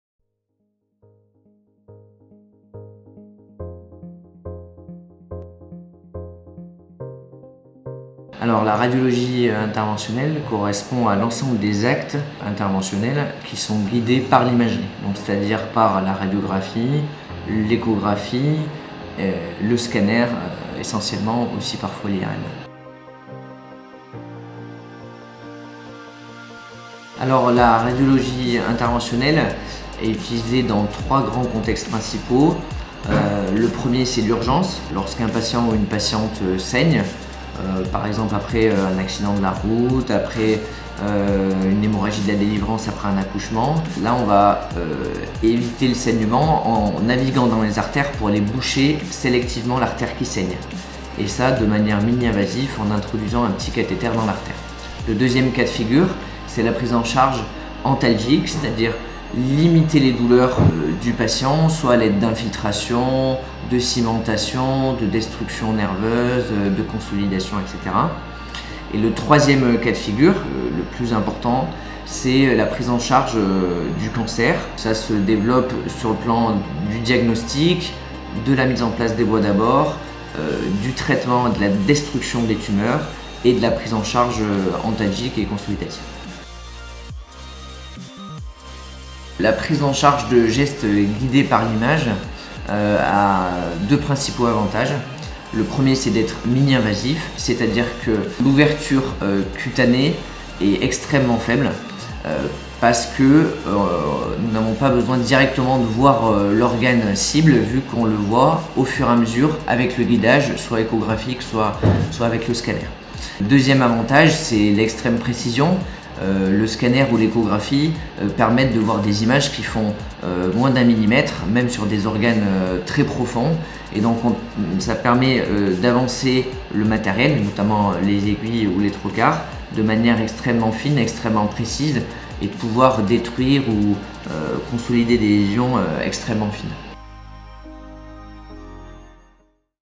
La Radiologie Interventionnelle - Qu'est-ce que c'est ?
Présentation détaillée de la radiologie interventionnelle par le Dr Sylvain Grange, radiologue au CHU de Saint-Etienne.